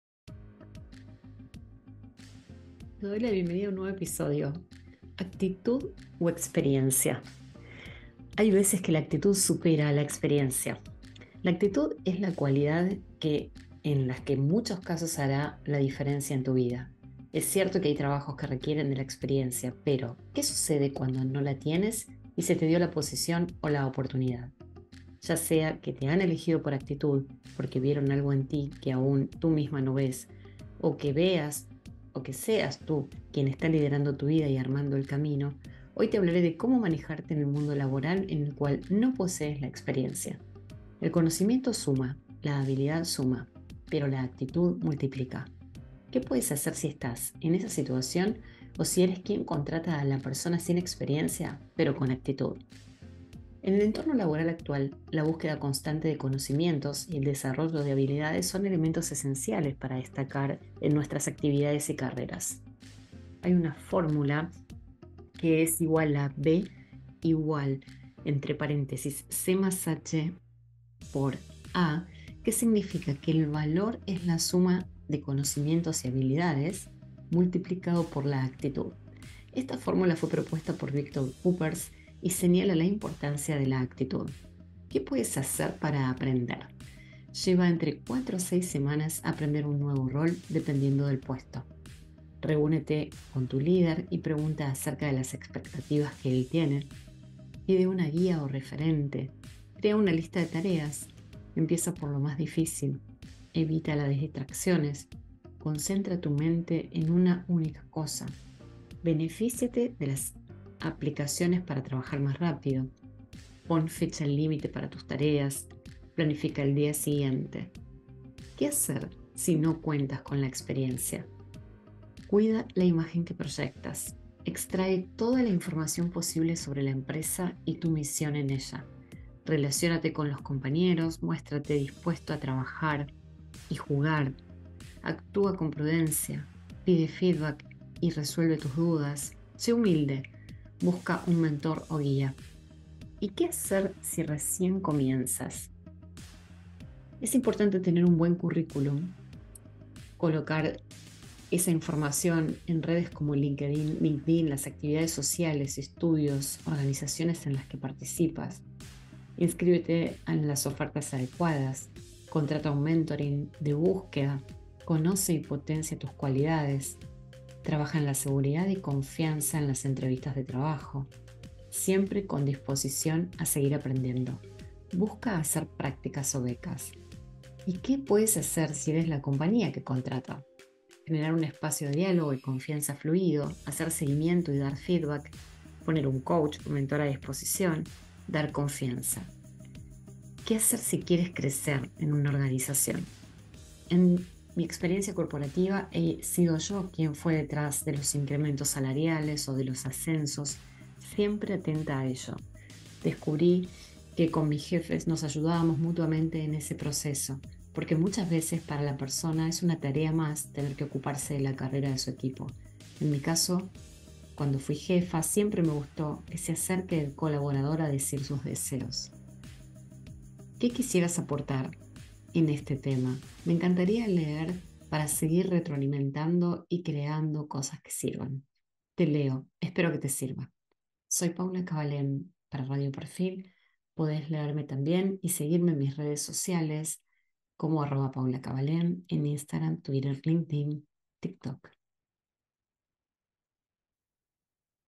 ¿Actitud o experiencia? Qué vale más a la hora de contratar o buscar trabajo.
Escribí este artículo para Revista Ellas de Panamá y para mi podcast en Radio Perfil. Puedes verlos en la sección Articulos y Podcast de mi web.